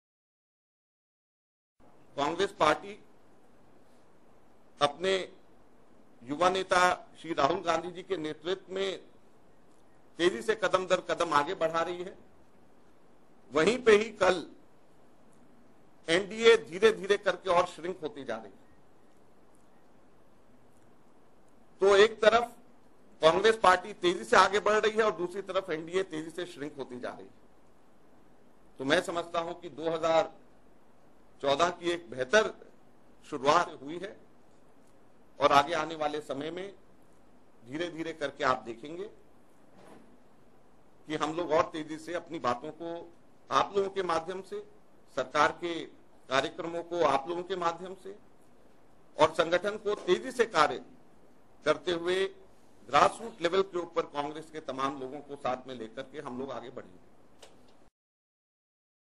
For Maken, shrinking of NDA is good start for Congress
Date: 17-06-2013

Congress General Secretary Ajay Maken has said that the shrinking of NDA is a good indication for Congress in the coming election.

He was addressing media persons after getting the new assignments by Congress recently.